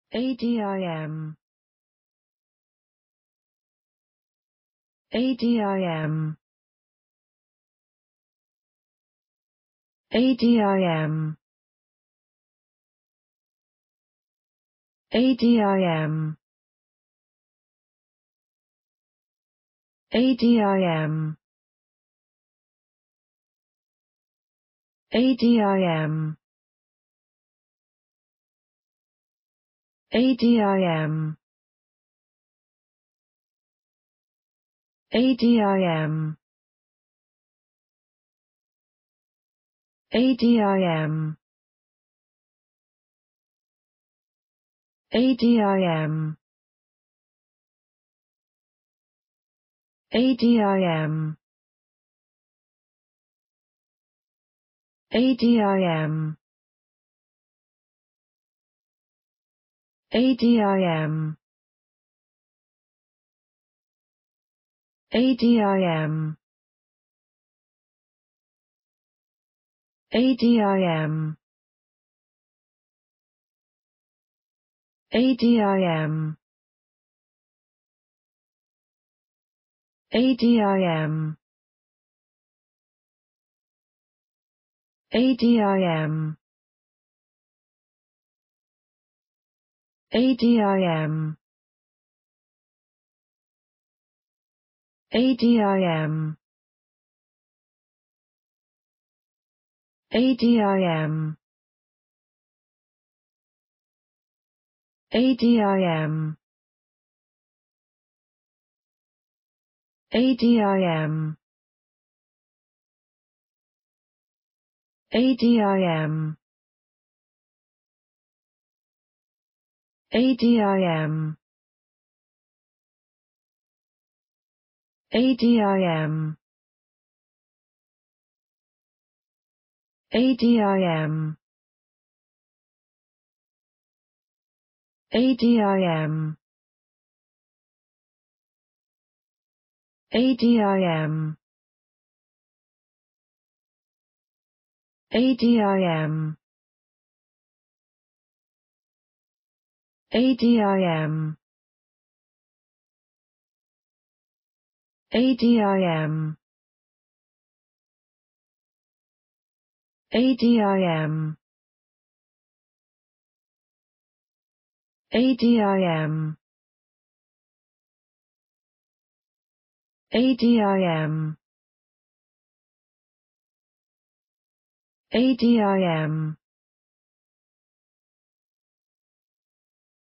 Amazing resource.Learn from me how to say words in English and pronounce the english word ADIM
Follow me!Learn from me how to pronounce the english word ADIM.This video shows you how to pronounce the English word  ADIM.